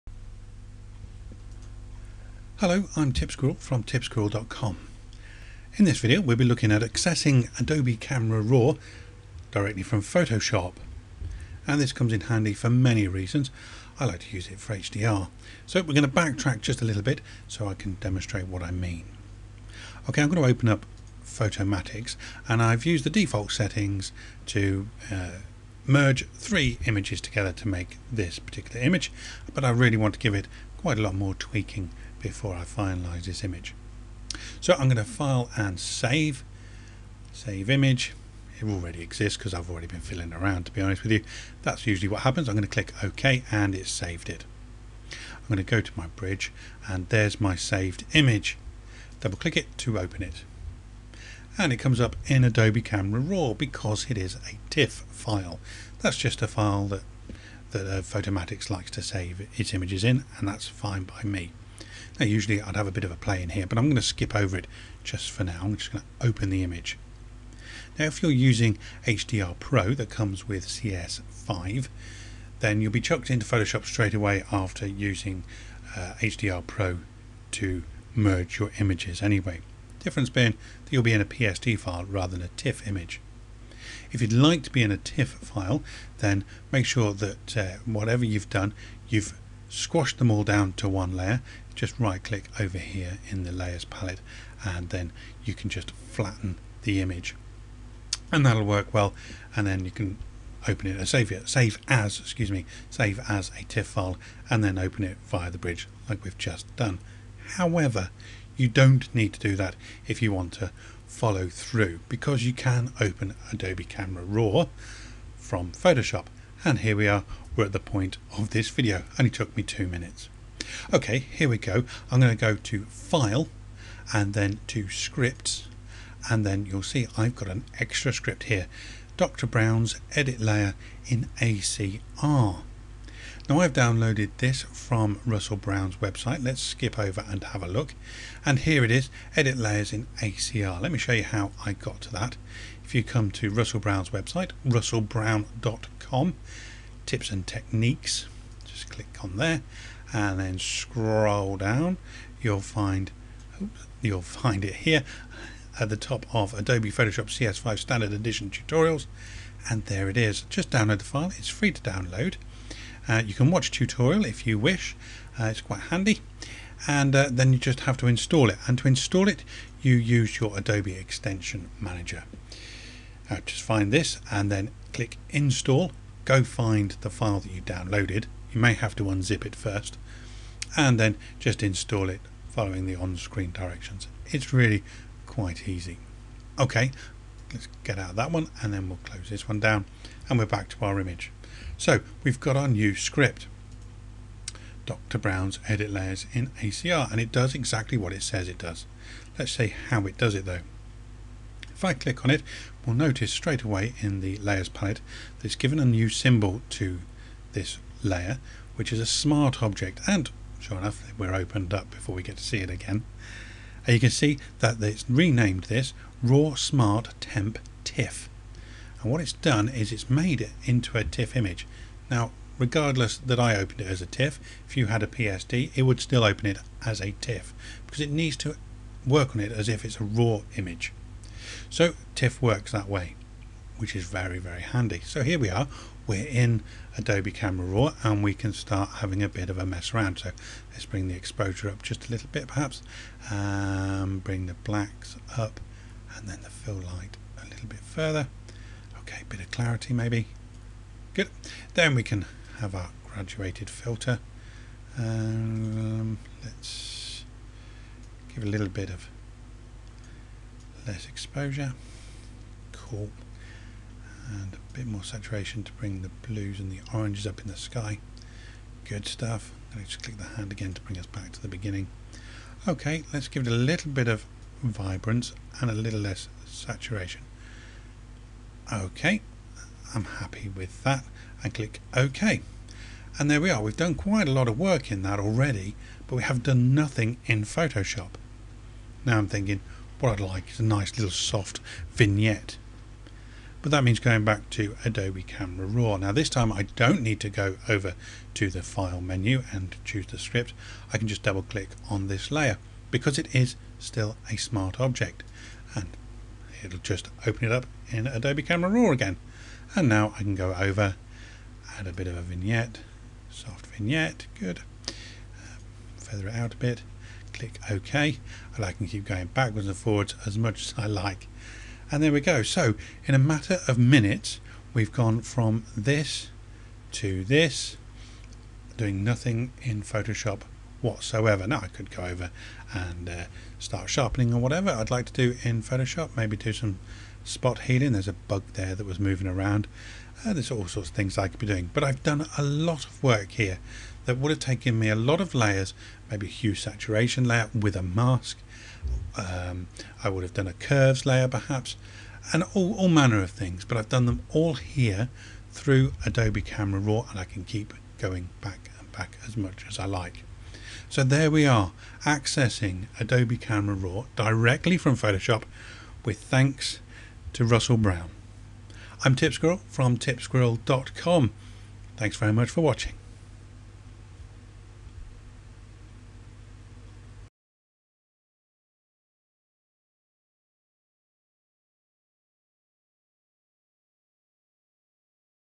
Accessing Adobe Camera Raw from Photoshop
Adobe Camera Raw is a great way to edit images fast, so wouldn't it be great if you could access it straight from Photoshop? Well you can!